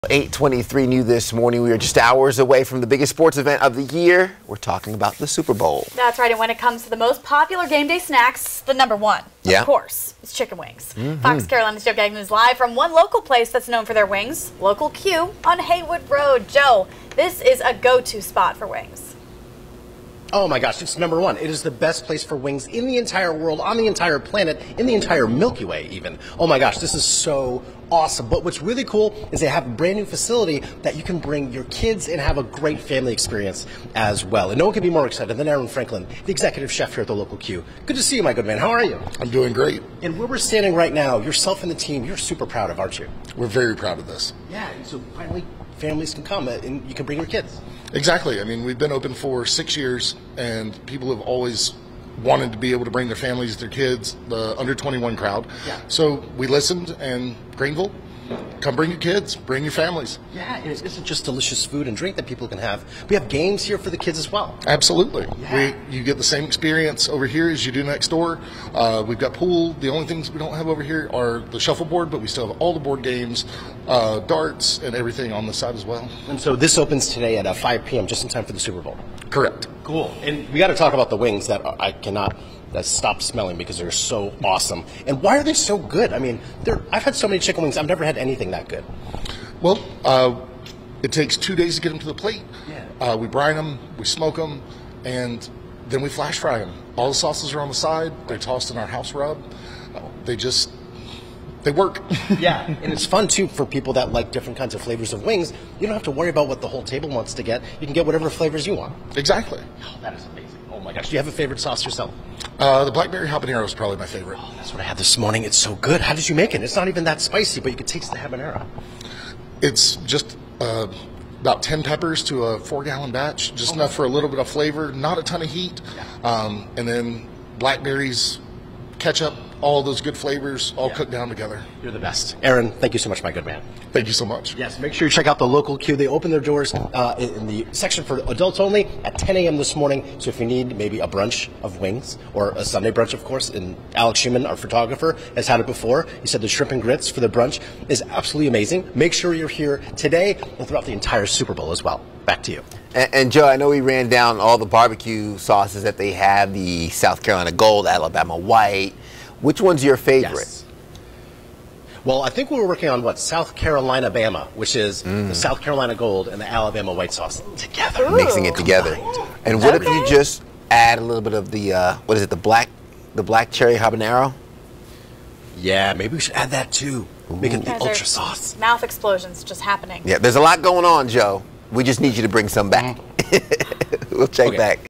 Game day wings at Local Cue
Joe is live at Local Cue, known for their wings, for the biggest game day of the year.